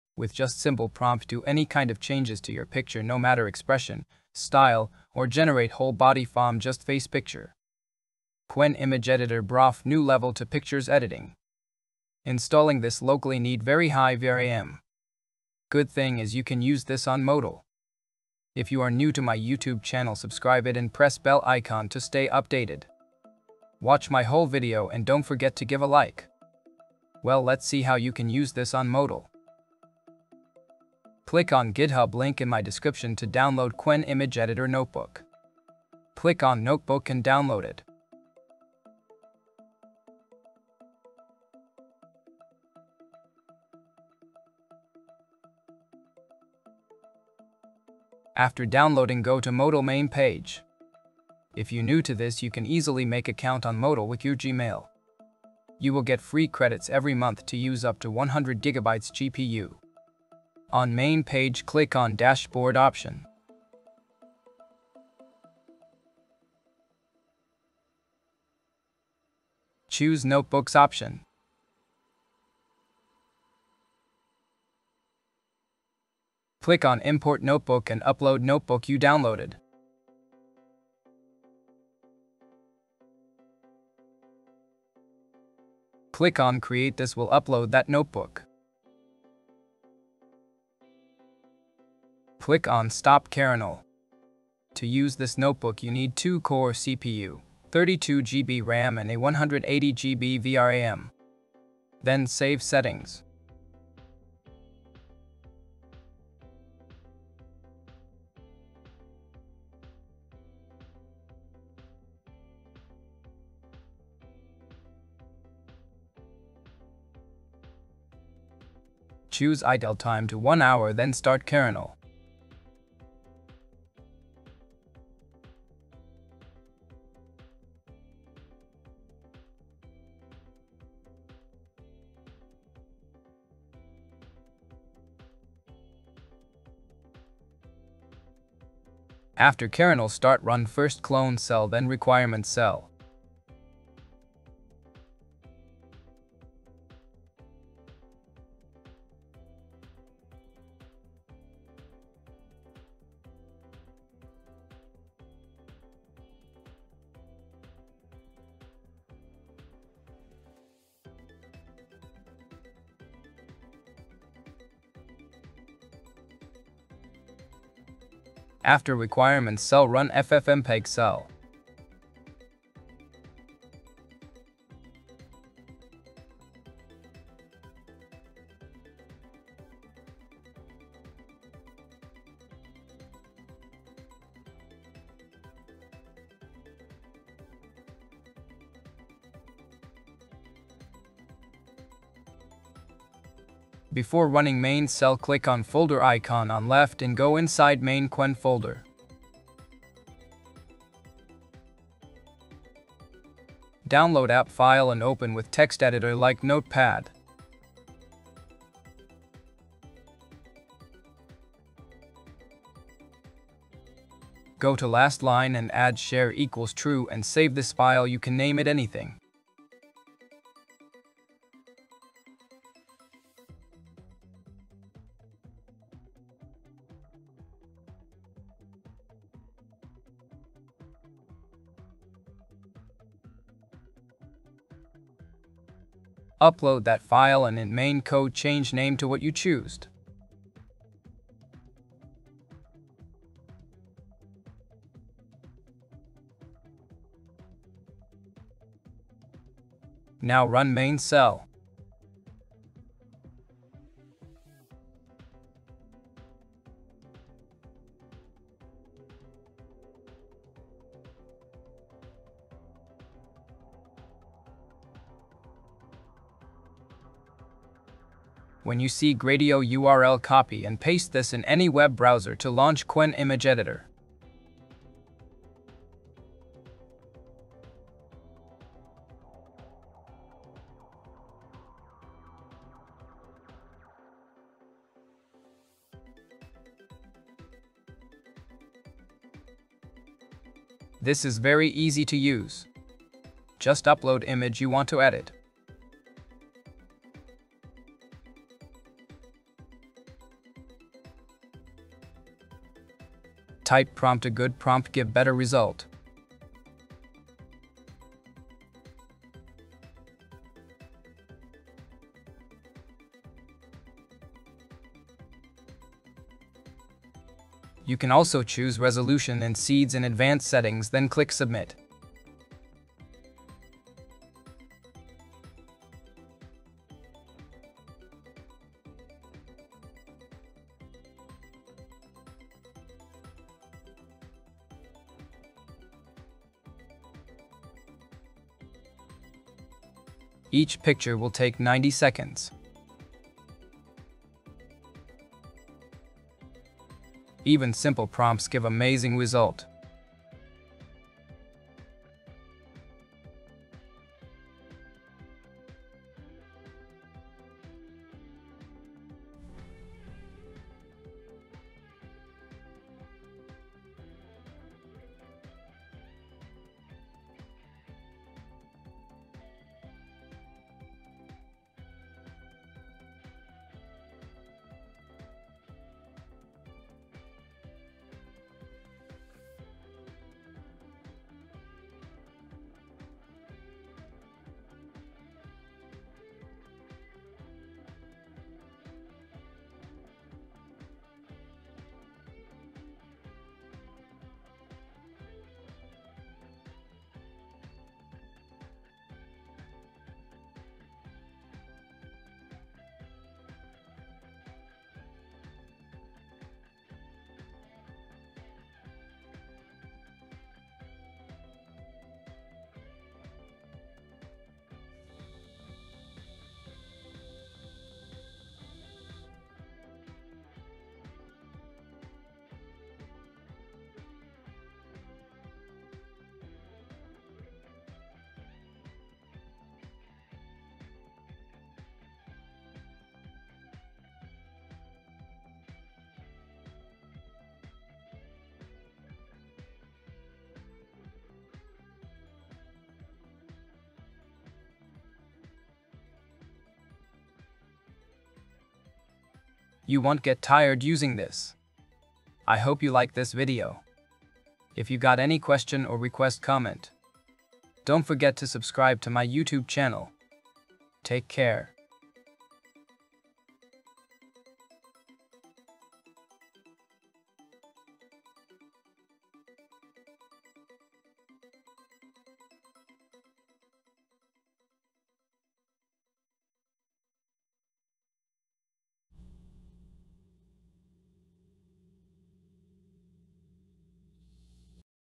"Qwen Image Editor on Modal | 100GB Free GPU to Edit Pose, Expression & Create Full Body"
Edit your photos like never before with Qwen Image Editor! 🎨
With just a simple text prompt, you can:
✅ Change poses & expressions
✅ Apply different styles & effects
✅ Generate a full body from just a face photo

The best part? 💻 You don’t need high VRAM to run it locally – use it on Modal with 100GB Free GPU and start editing instantly!